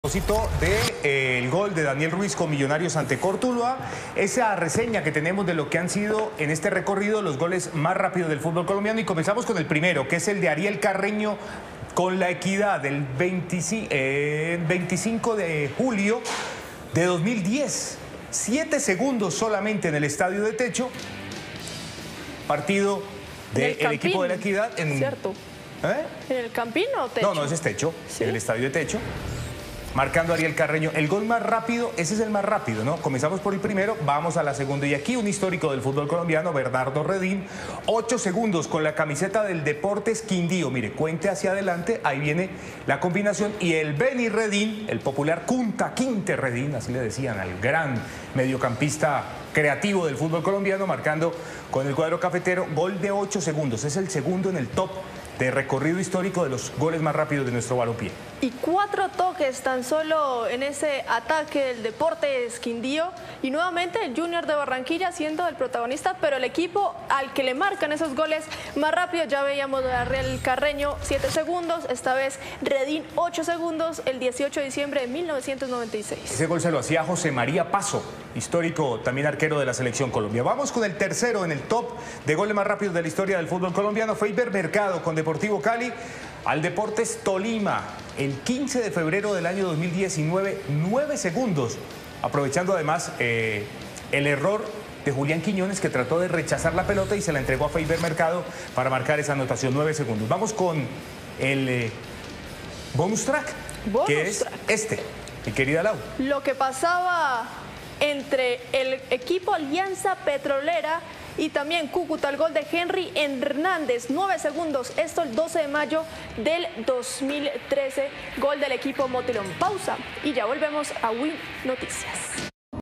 ¡Inolvidables! Los goles más rápidos de la historia del Fútbol Colombiano
Ariel Carreño, con Equidad, posee el récord absoluto.